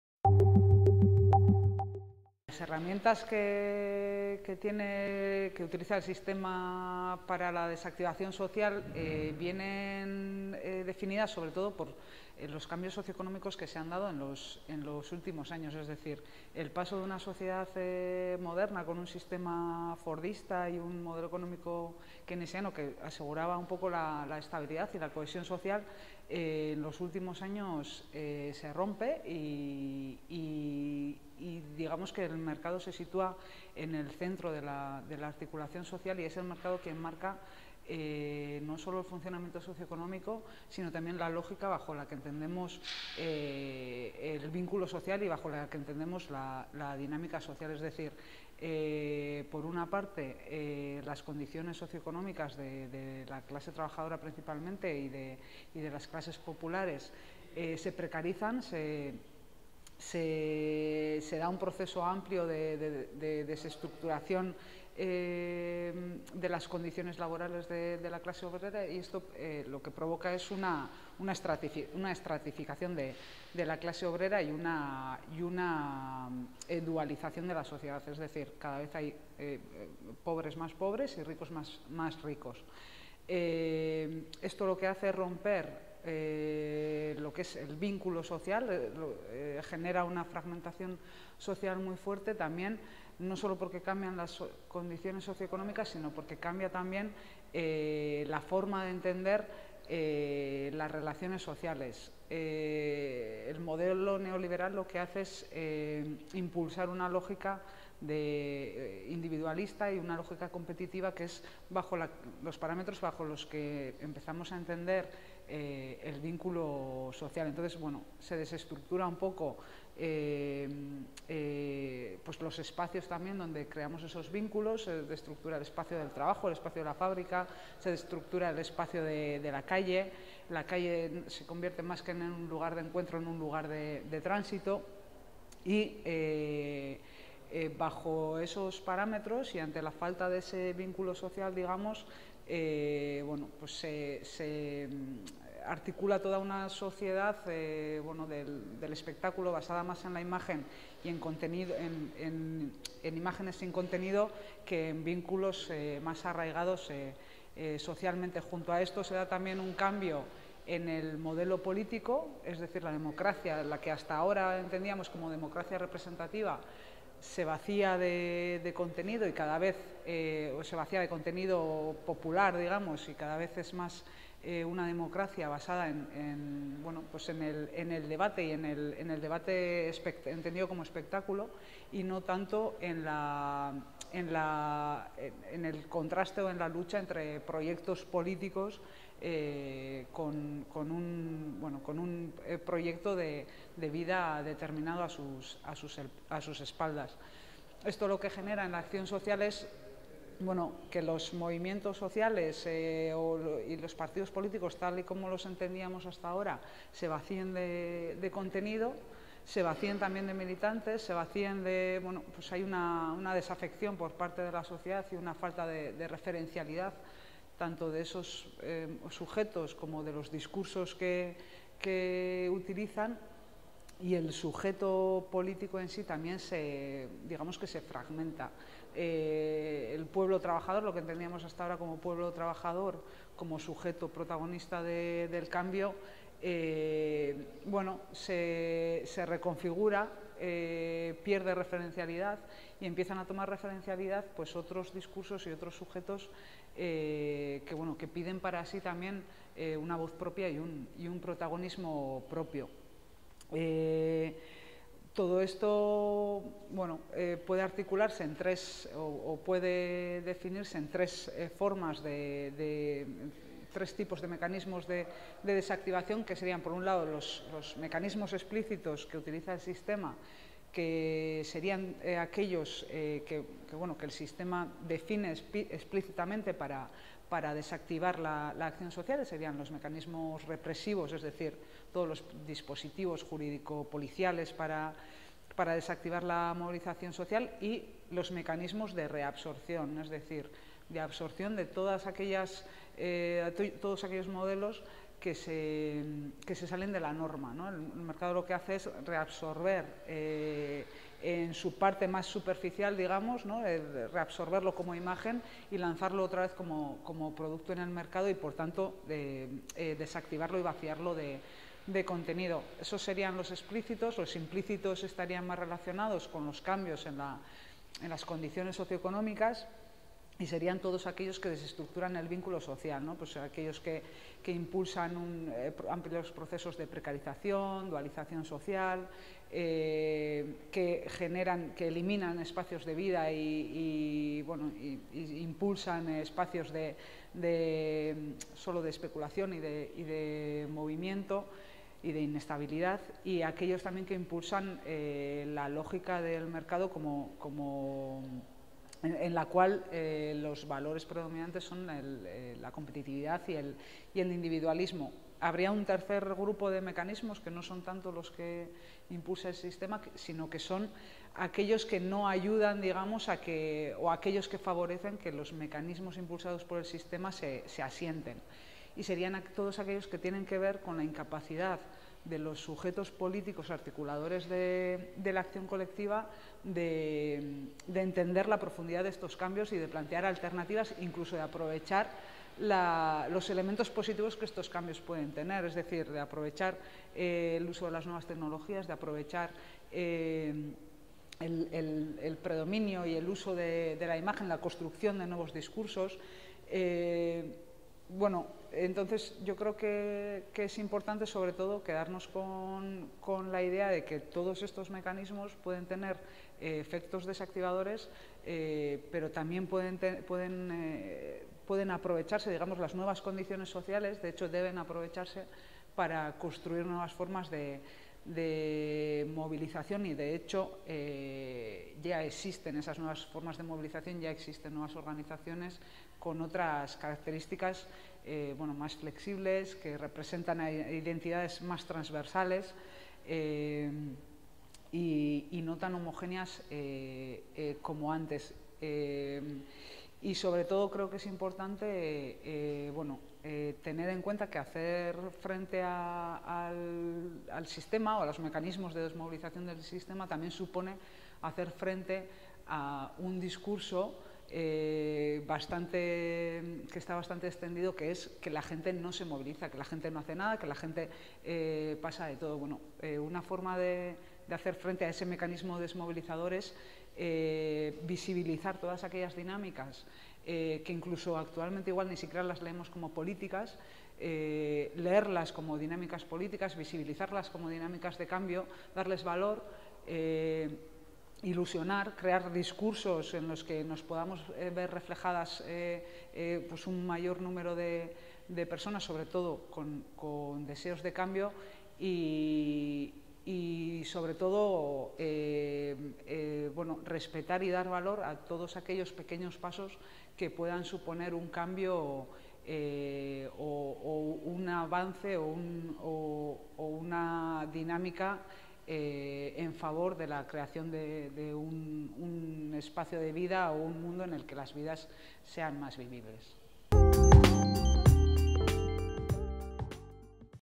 Sandra Barrenetxea | Sandra Barrenetxea Herramientas del sistema para la desactivación social
Eraldaketa prozesua eta aktibazio soziala [Bir]Pentsatzen jardunaldiak burutu genituen  2016ko ekainaren 15 eta 16 egunetan Berriozarren.

Krisiaren ondorioak subjektu aktiboa eratzeko gaia hartuta aritu ziren Eduardo Apodaka eta Sandra Barrenetxea